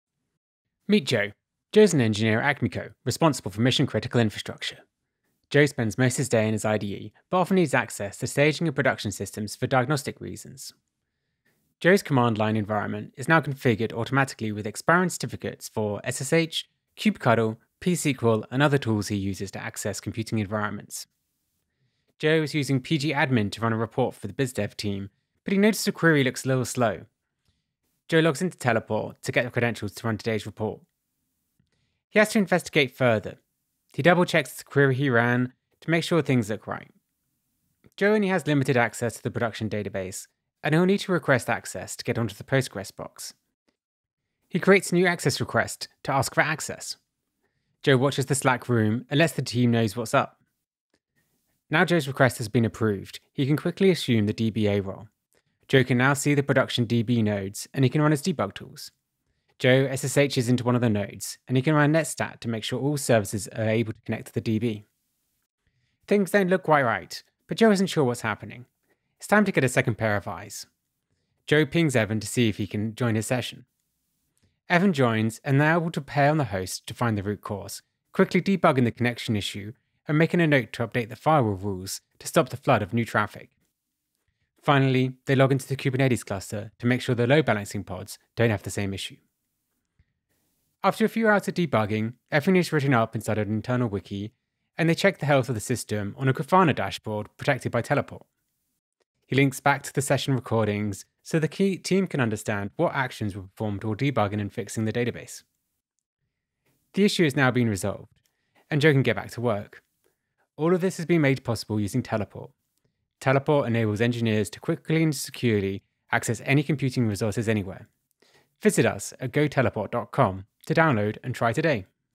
Teleport Demo
Teleport enables engineers to quickly access any computing resource anywhere.

A day in the life of Joe.

0:04 tsh login
0:23 Teleport Database Access
0:30 Teleport Audit Log
0:38 Access Requests
0:46 Teleport Slack Plugin
0:53 Assume Role
1:11 Sharing a SSH Session
1:24 Teleport Kubernetes Access
1:36 Teleport Application Access
1:41 Session Recording
1:59 Download Now at